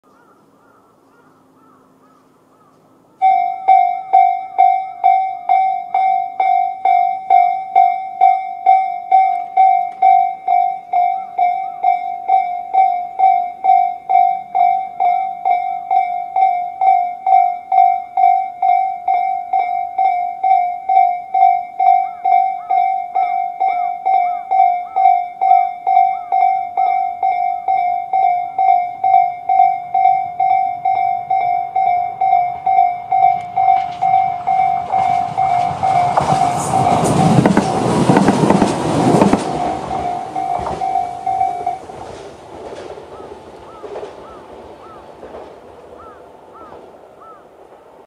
東武小泉線(館) 第23号踏切道(その3)
←成島　×小(館)　第23号踏切道×　本中野→
場所：群馬県館林市成島町

成島駅から本中野駅に向かって4箇所目の踏切です。
音は両側共に東武音Cです。

・通過車両
1：739E列車　8000系8572F

・警報灯
北側1：日本信号株式会社［閃光灯　EP1506F形：07年10月製造］
　　2：日本信号株式会社［閃光灯　EP1506F形：07年10月製造］
南側1：日本信号株式会社［閃光灯　EP1506F形：07年10月製造］
　　2：日本信号株式会社［閃光灯　EP1506F形：07年10月製造］

・遮断機
北側：日本信号株式会社［電気踏切しゃ断機　EM1029A形：08年02月製造］
南側：日本信号株式会社［電気踏切しゃ断機　EM1029A形：08年02月製造］

成島駅
↕ 0.2km
小(館)　第14号踏切道
↕ 0.3km
小(館)　第17号踏切道
↕ 0.4km
小(館)　第20号踏切道
↕ 0.8km
小(館)　第23号踏切道
↕ 0.3km
小(館)　第24号踏切道
↕ 2.0km
小(館)　第29号踏切道
↕ 0.1km
本中野駅